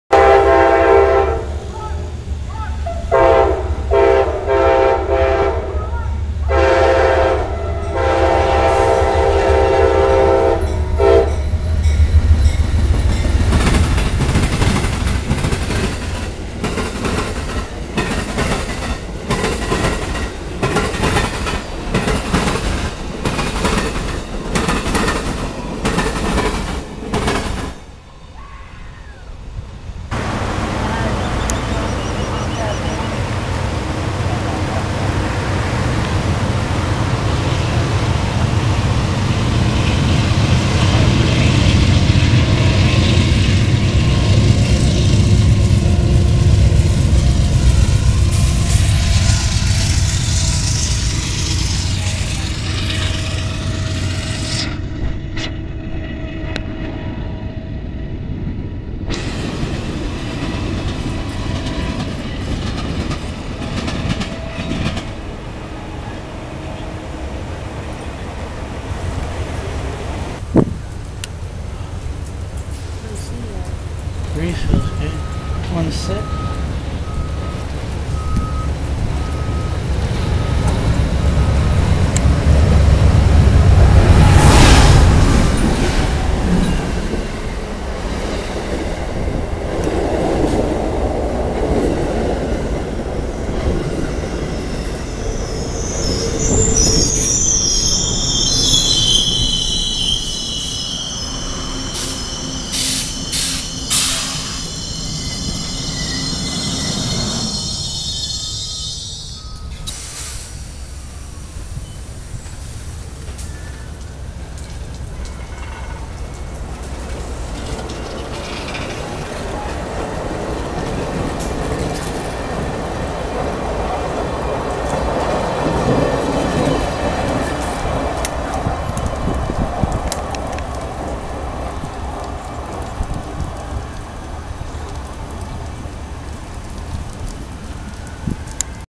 Amtrak Train The Silver Stars Rolling Around
Hello This Is A Video I Made Of The Amtrak Train The Silver Stars Rolling Around Town.